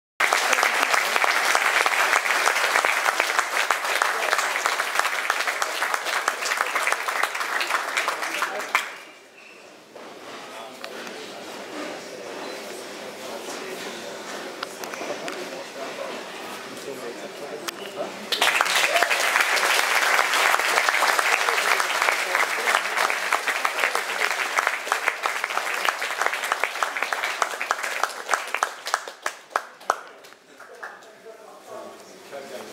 RGA Grading - Yas Wilson receives Brown Belt
Yas Wilson received brown belt from Roger Gracie